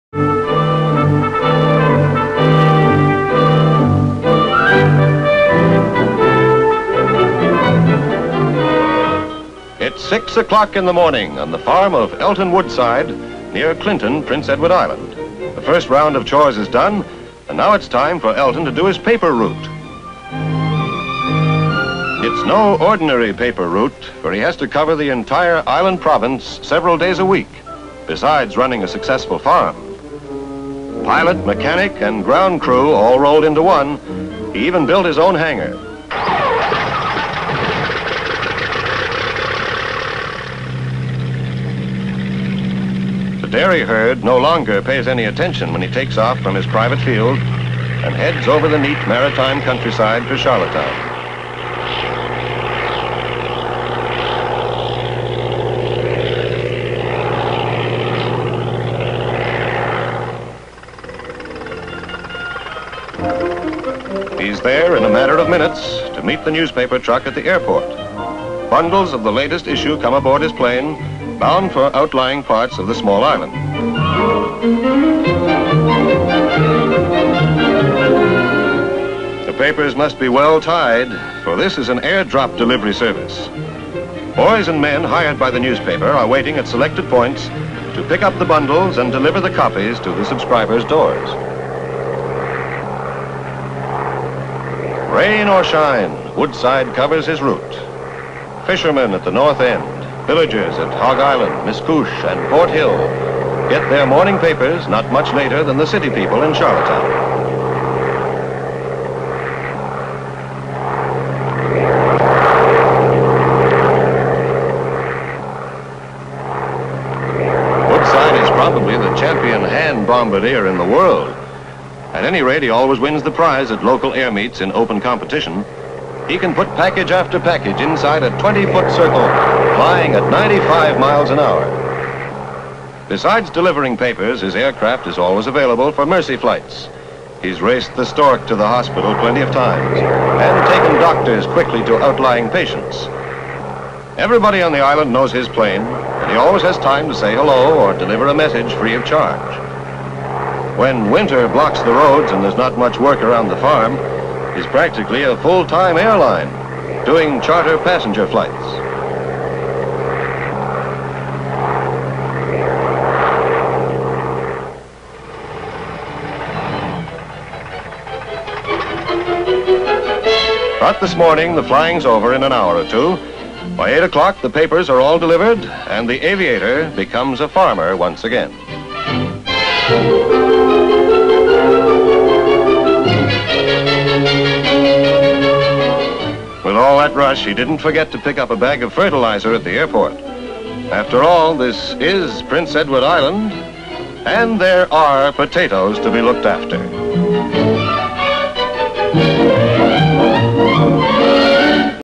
Flying Farmer 1952
Elton Woodside of Clinton, Prince Edward Island, was known as the "Flying Farmer" after earning his pilot's licence in 1946 and beginning frequent flights over the Island in his Fleet Canuck aircraft while still operating his farm. By the mid 1950s, Woodside was flying The Guardian newspaper from Charlottetown to carriers in western P.E.I. daily, as well as making twice weekly flights to Nova Scotia for the mail. Woodside was also credited with saving lives and rushing expectant mothers to the hospital.